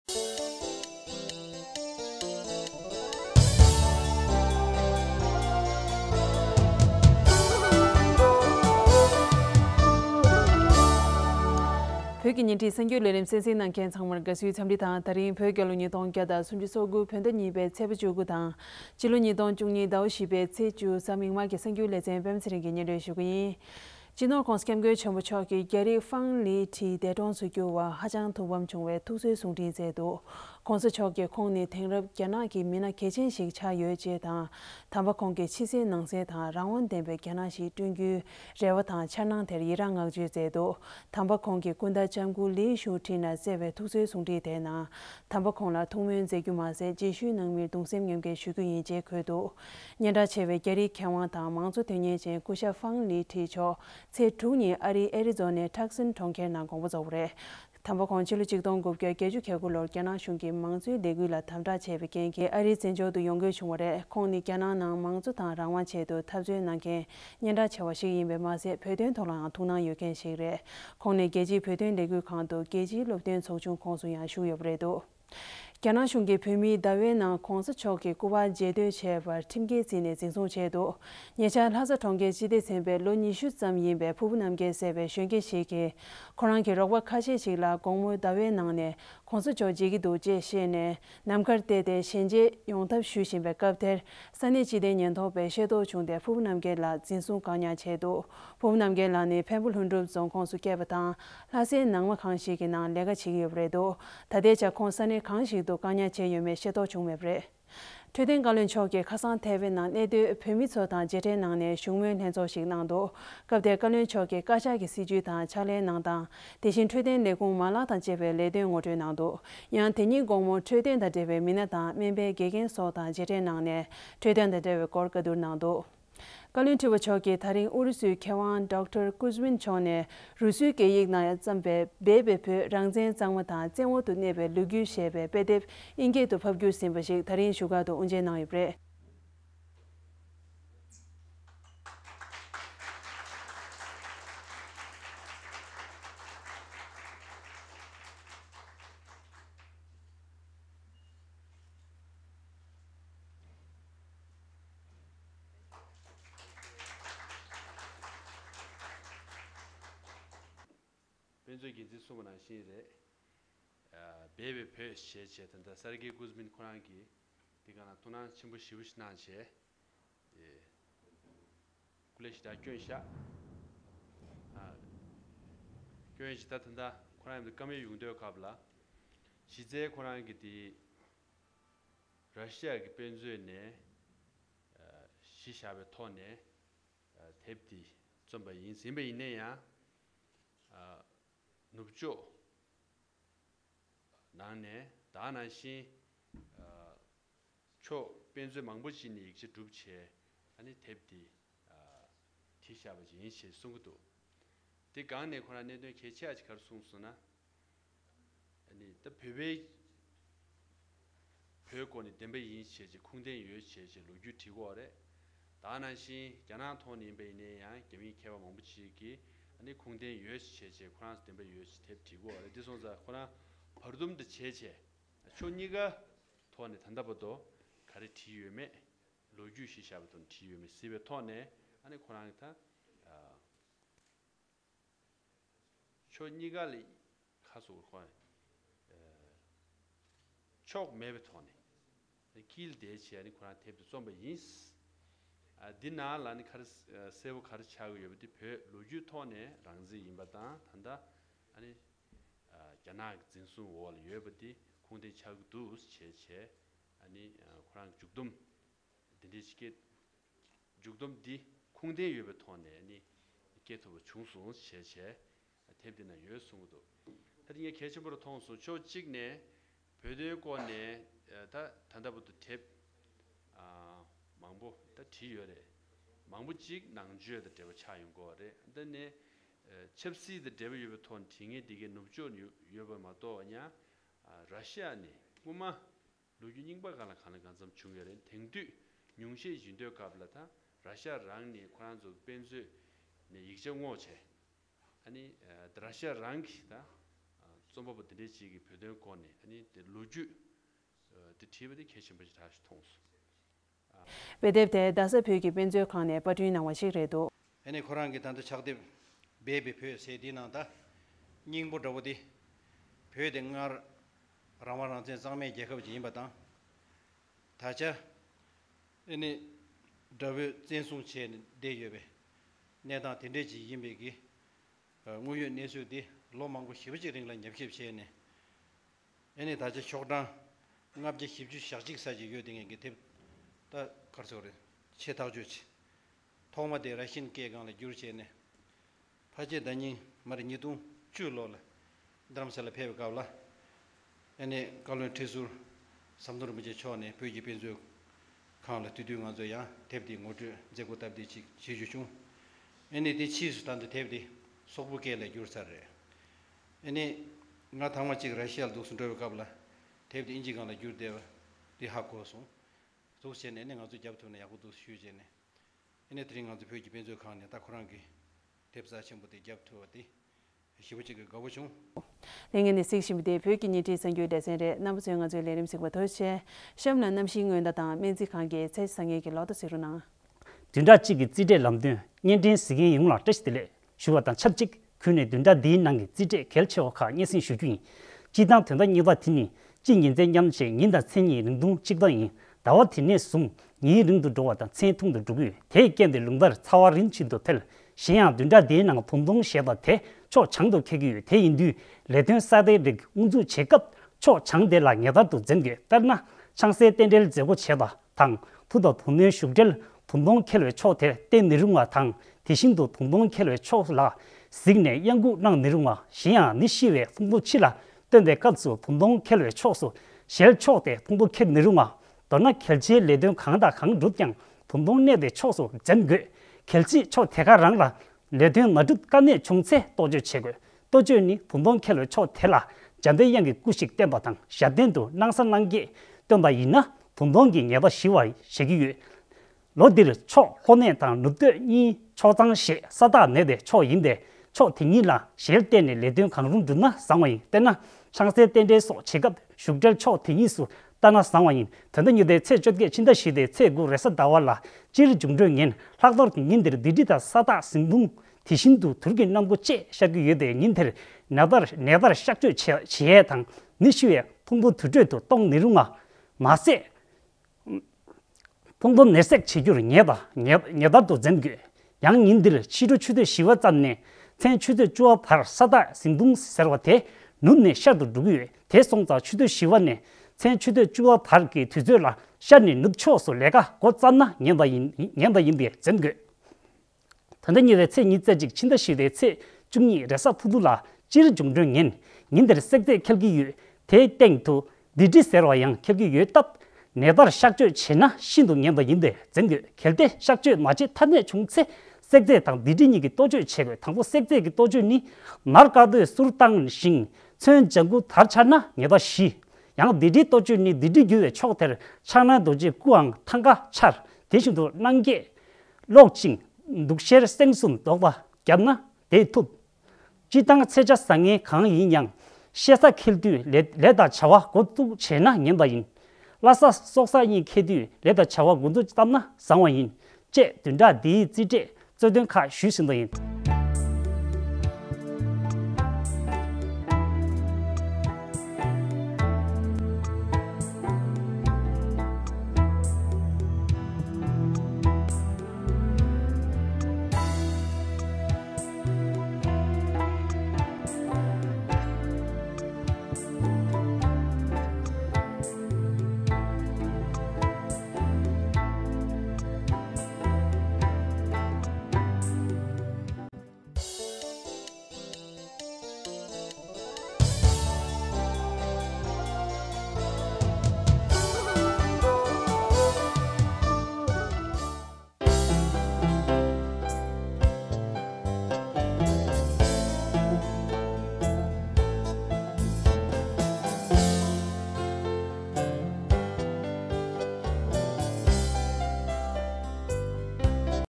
10 Apr. 2012 - Tibetonline.tv News
Headlines: His Holiness Condoles Demise of Fang Lizhi ◆ China Arrested a Tibetan Youth in 20s ◆ Kalon Tsering Wangchuk Meets Tibetans in Taiwan ◆ Kalon Tripa Releases a Book Titled 'Hidden Tibet: History of Independence & Occupation' ◆